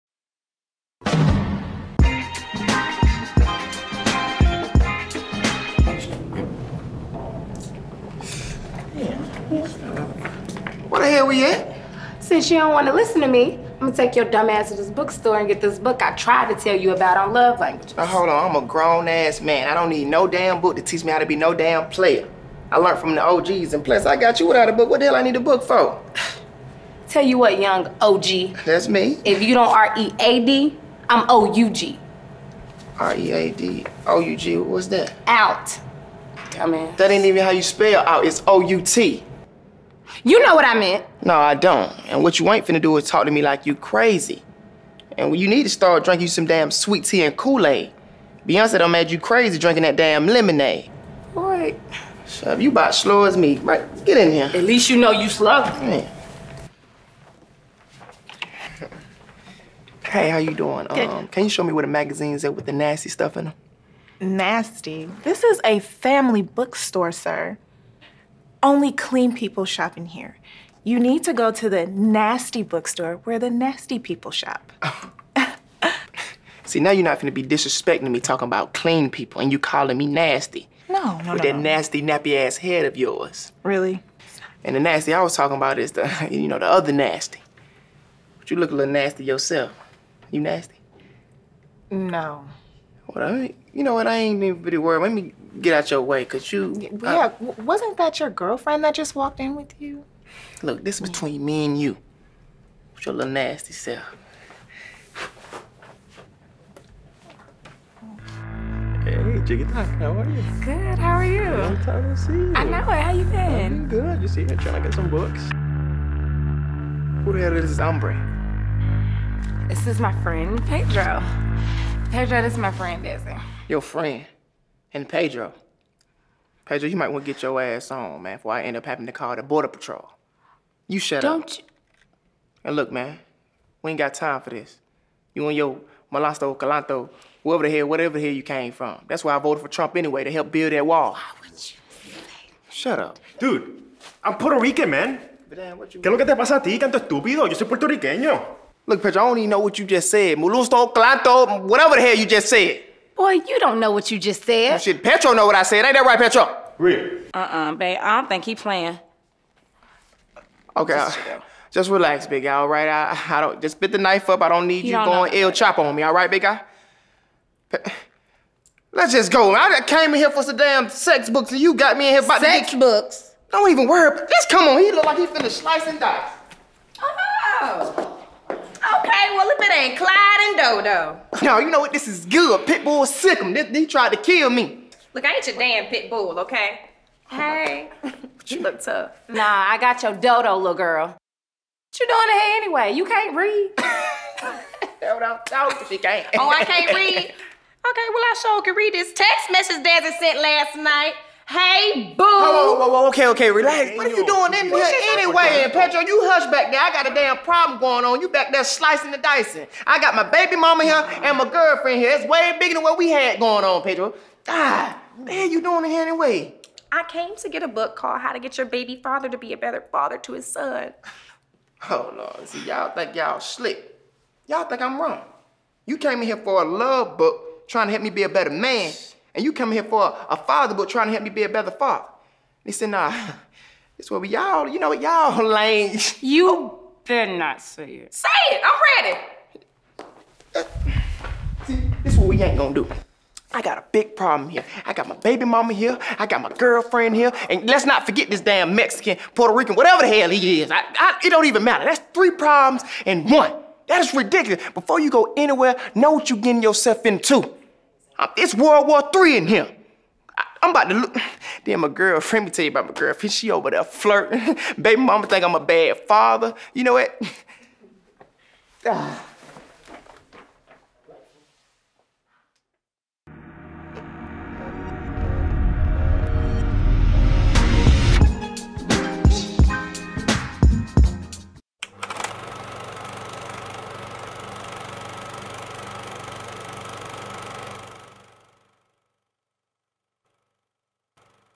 Baby Mama Drama in the Bookstore
World War III with your girlfriend and baby mama